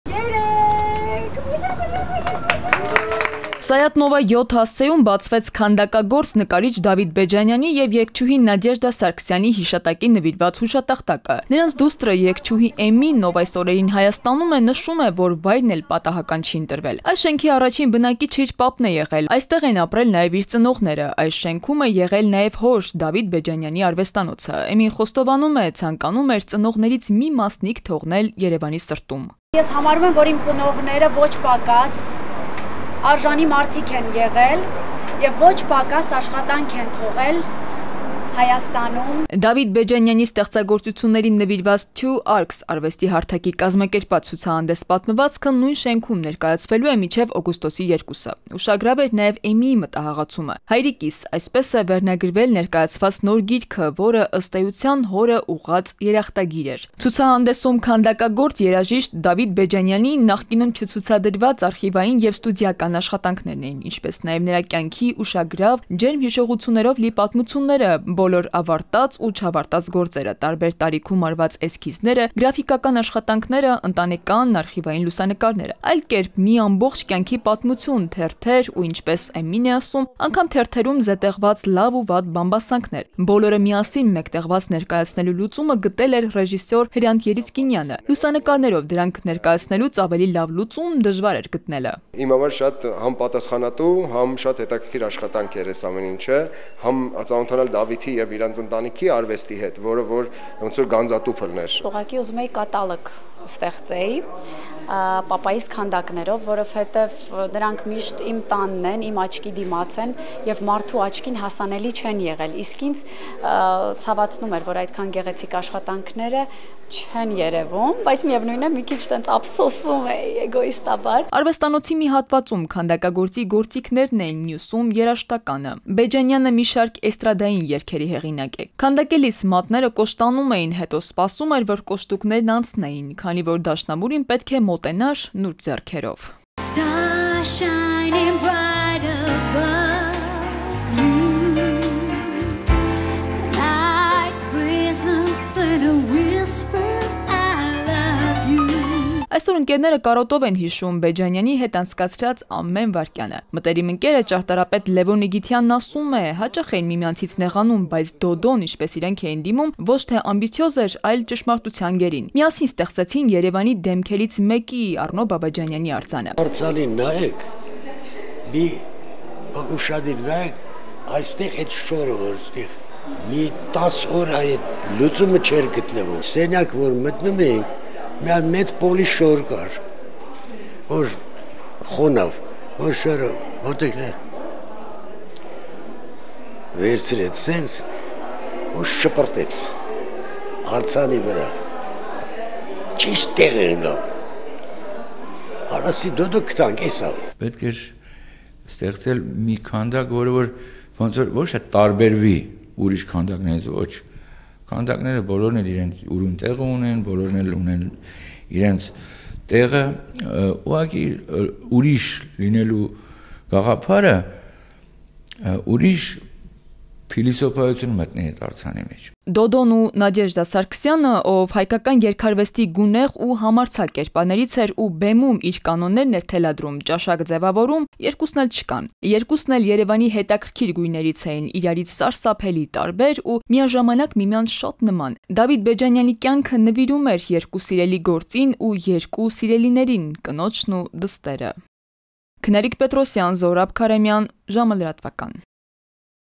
ՔԱՆԴԱԿԱԳՈՐԾ ԴԱՎԻԹ ԲԵՋԱՆՅԱՆԻ ԵՎ ԵՐԳՉՈՒՀԻ ՆԱԴԵԺԴԱ ՍԱՐԳՍՅԱՆԻՆ ՆՎԻՐՎԱԾ ՀՈՒՇԱՏԱԽՏԱԿ՝ ՍԱՅԱԹ ՆՈՎԱ 7-ՈՒՄ
Երևանում բացվել է Դավիթ Բեջանյանի ու Նադեժդա Սարգսյանի անվան հուշատախտակ: Հուշատախտակը բացել են նրանց թոռնիկները` երգչուհի Էմմիի որդիները` Դանիելն ու Մարտինը։